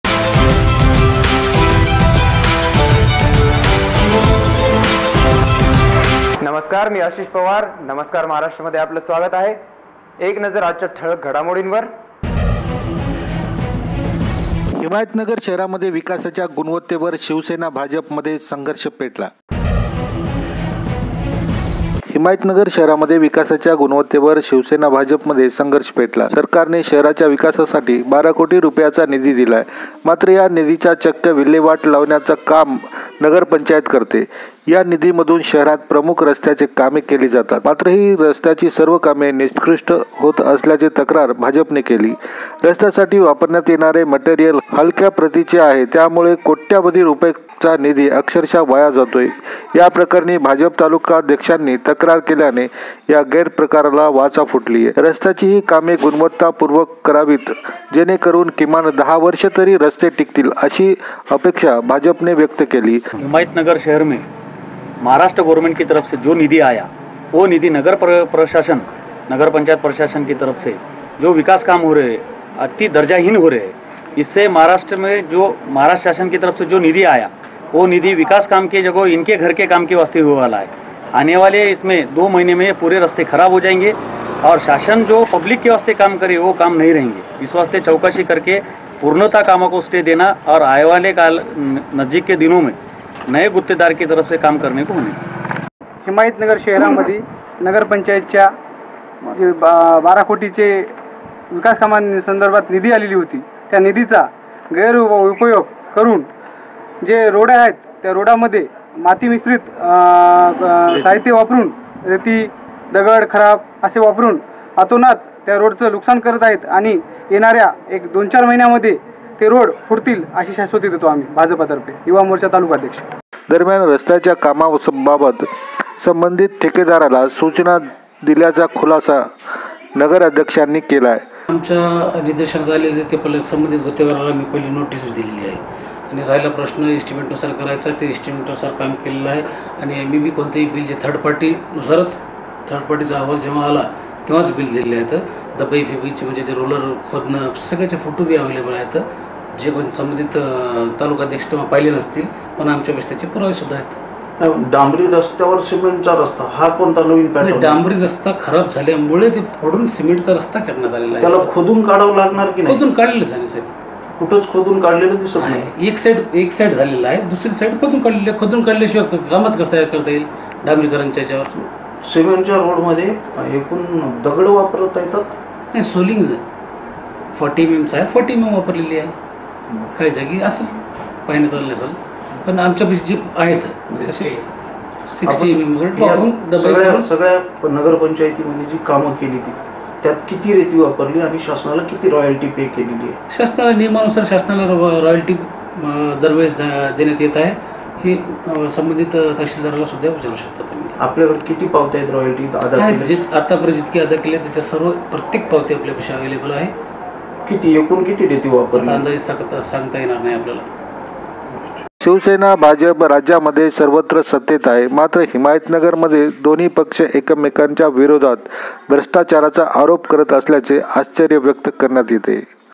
हिमायतनगर शहरामध्ये विकासाकामाच्या गुणवत्तेवरून शिवसेना भाजप मध्ये संघर्ष पेटलाय
हिमायतनगर शहरामध्ये विकासाकामाच्या गुणवत्तेवरून शिवसेना - भाजप मध्ये संघर्ष पेटलाय. सरकारने शहराच्या विकासासाठी 12 कोटी रुपयाचा निधी दिलाय. मात्र या निधीची चक्क विल्हेवाट लावण्याच काम नगर पंचायत करतेय. या निधीमधून शहरात प्रमुख रस्त्याची कामे केली जातायत. मात्र ही रस्त्याची ही सर्व कामे निकृष्ट होत असल्याची तक्रार भाजपने केलीय. रस्त्यासाठी वापरण्यात येणारे मटेरियल हलक्या प्रतीचे आहे. त्यामुळे कोट्यवधी रुपयाचा निधी अक्षरक्ष वाया जातोय. या प्रकरनी भाजप तालुकाध्यक्षानी तक्रार केल्याने या गैरप्रकाराला वाचा फुटलीय. रस्त्याची ही कामे गुणवत्ता पूर्वक करावीत जेणेकरून किमान दहा वर्षे तरी रस्ते टिकतील अशी अपेक्षा भाजपने व्यक्त केलीय. दरम्यान रस्त्याच्या कामाबाबत संबंधित ठेकेदाराला सूचना दिल्याचा खुलासा नगर अध्यक्षांनी केलाय. शिवसेना भाजप राज्या मध्ये सर्वत्र सत्तेत आहे, मात्र हिमायत नगर मध्ये दोन्ही पक्ष एकमेकांच्या विरोधात भ्रष्टाचाराचे आरोप करत असल्याने आश्चर्य व्यक्त करण्यात येतेय